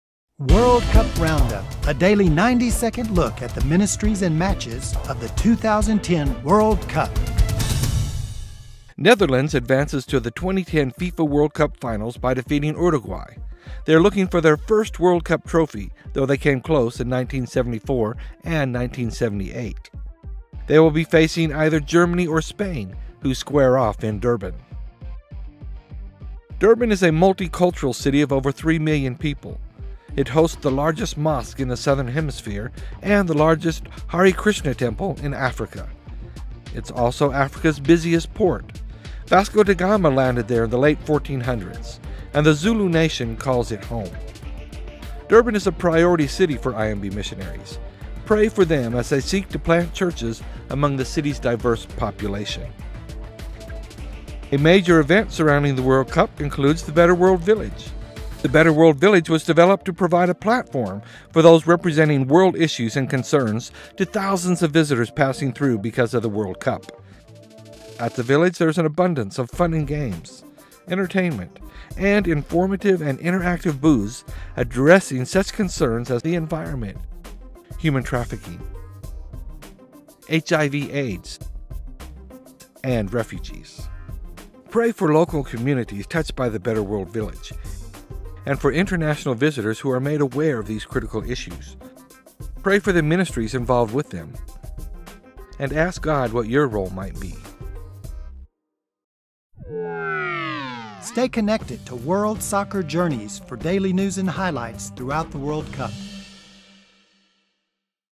World Cup Roundup: Episode 25
In this episode: Netherlands clinch their place in the World Cup final. Durban is another priority city for IMB missionaries. The Better World Village in Pretoria raises awareness about human trafficking, AIDS and refugees.